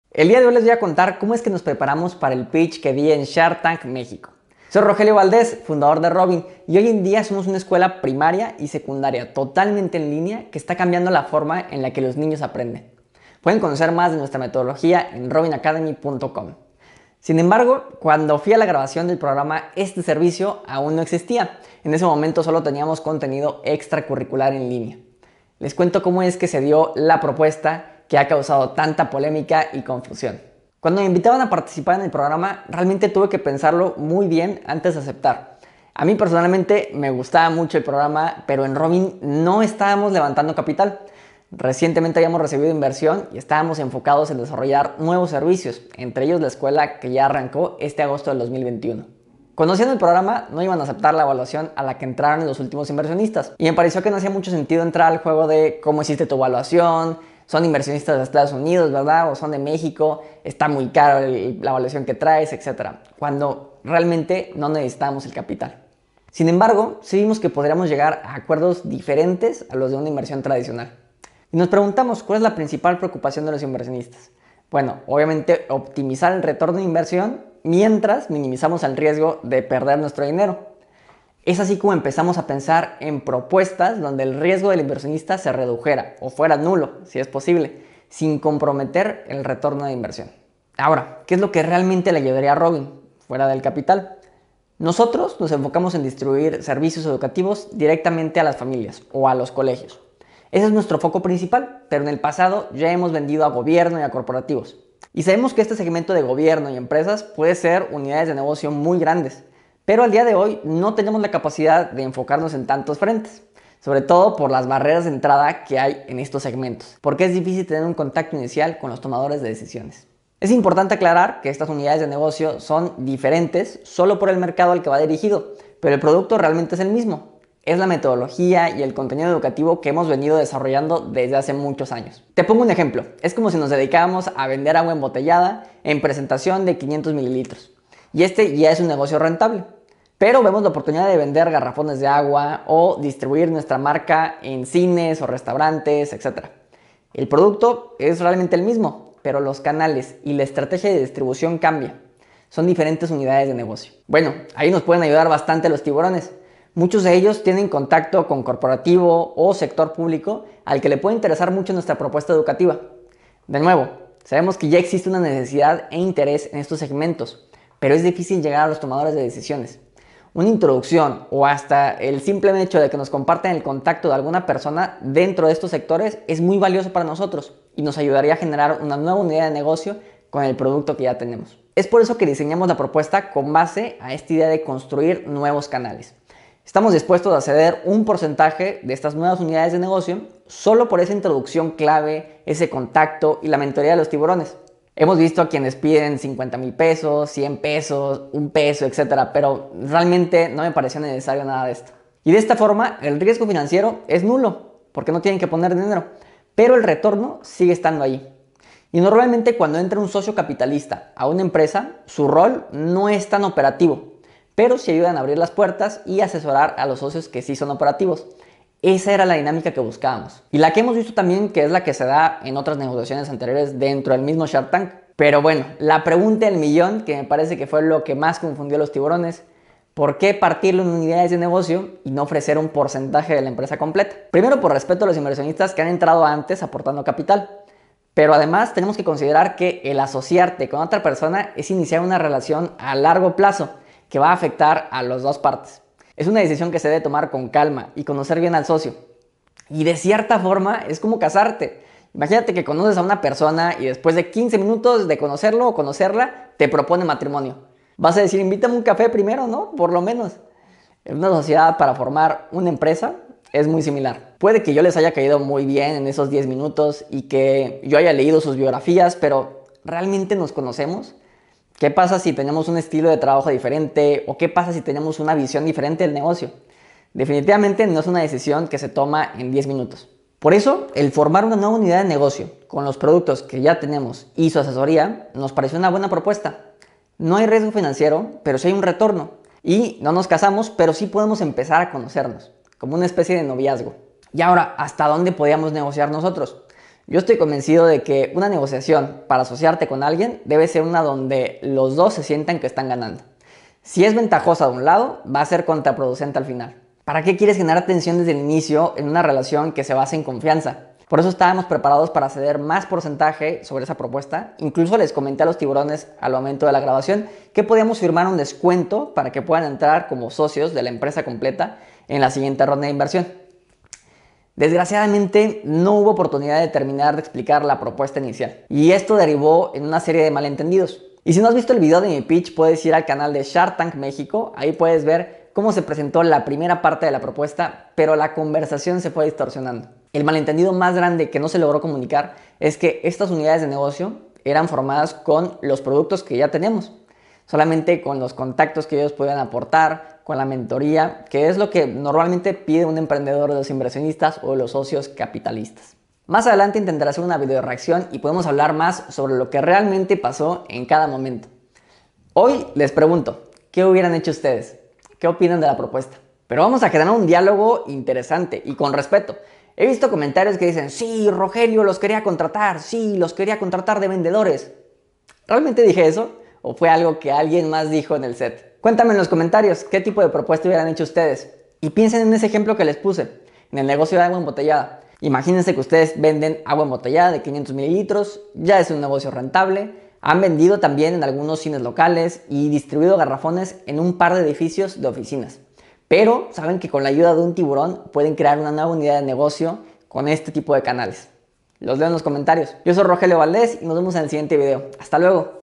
¿Quería Contratar a los Tiburones de SHARK TANK MEXICO? - Conoce la Propuesta detrás del Pitch
El día de hoy les voy a contar como es que nos preparamos para el pitch que di en Shark Tank Mexico. Soy Rogelio Valdes, fundador de Robin.

Hoy en día somos una escuela primaria y secundaria totalmente en línea que está cambiando la forma en la que los niños aprenden, pueden conocer más de nuestra metodología en robinacademy.com

Sin embargo cuando fue la grabación del programa este servicio aún no existía, en ese momento solo teníamos contenido extracurricular en línea. Les cuento como es que se dió la propuesta que ha causado tanta polémica y confusión...